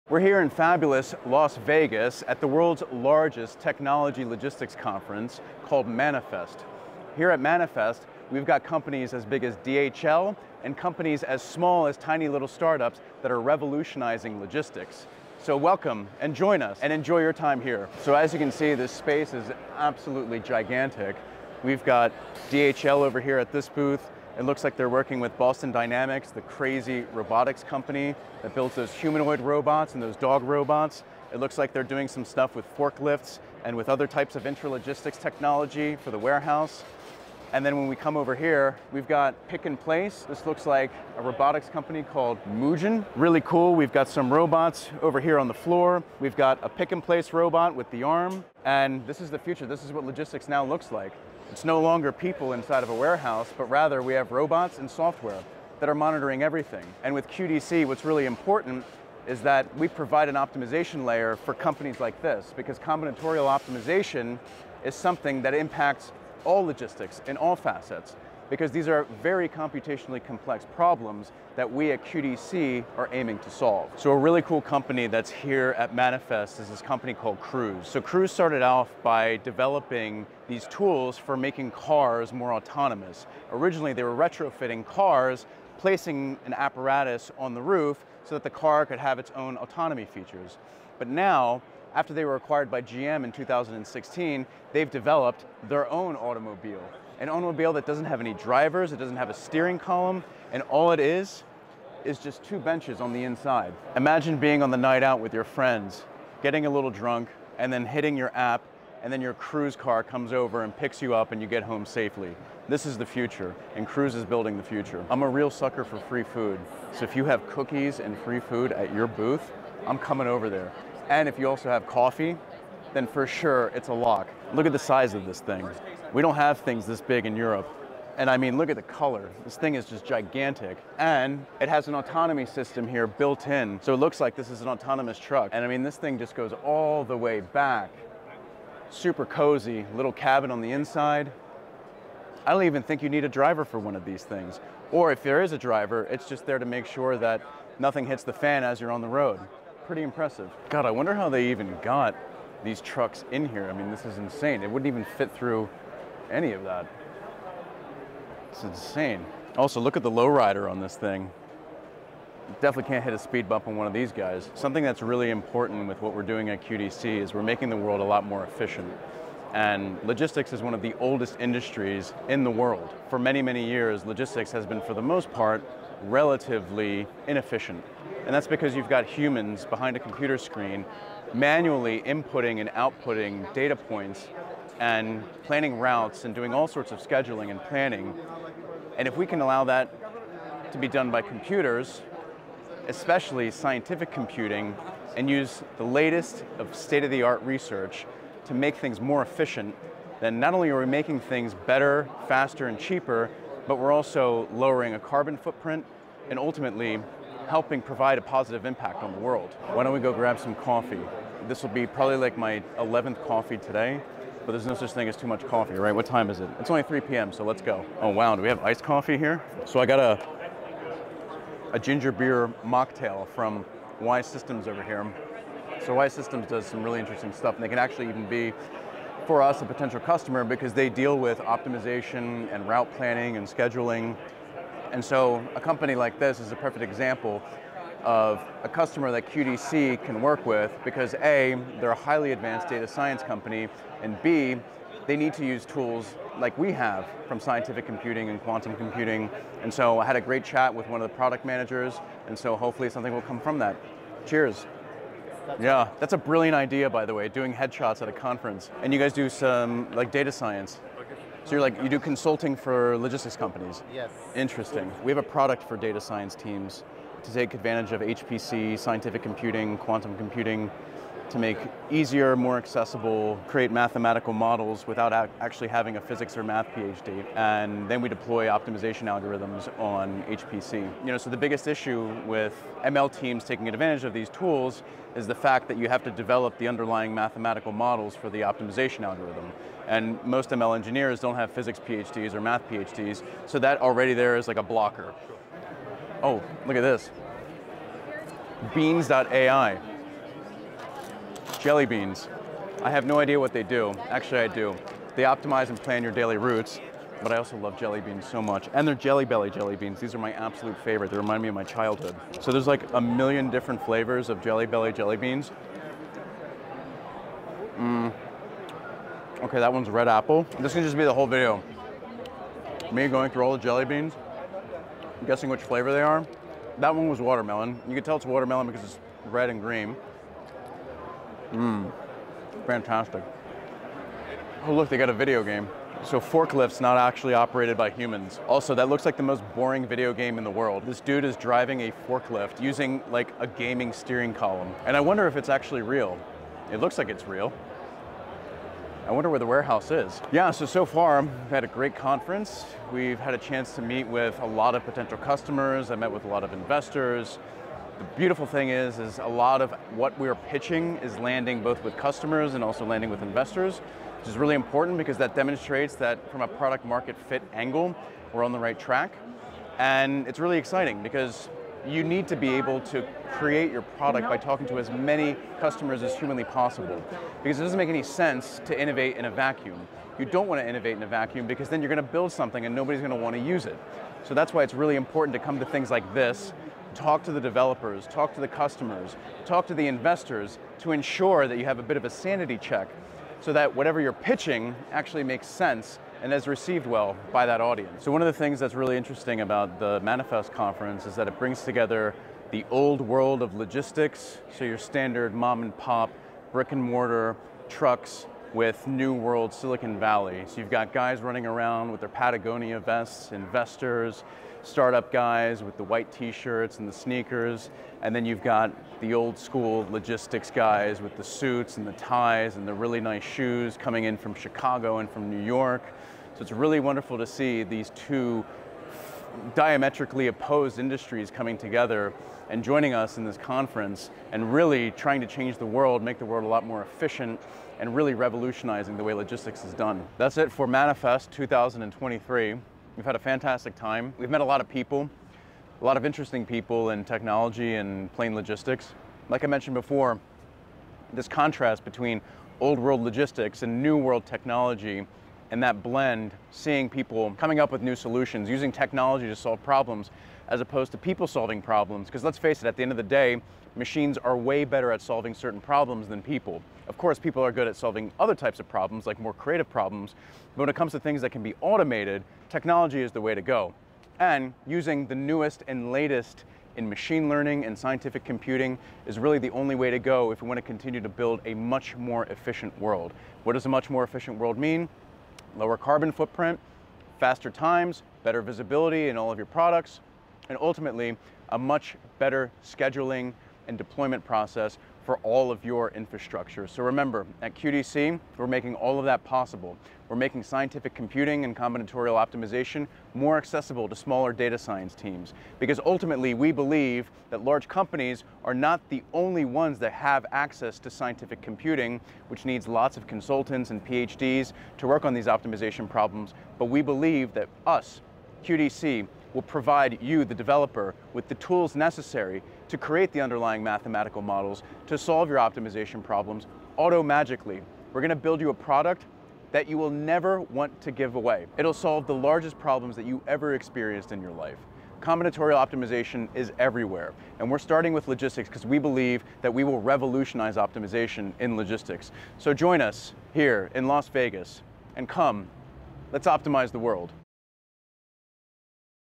Manifest 2023 Key Takeaways
I was at the world's largest logistics tech conference Manifest in Las Vegas, Nevada. It was great to see companies as large as DHL and as small as early stage startups showcasing how they're using the latest in technology to make the world a more efficient place. It was great to see our company's product received well amongst potential customers and investors.

At qdc.ai, we're making scientific computing more accessible to smaller data science teams.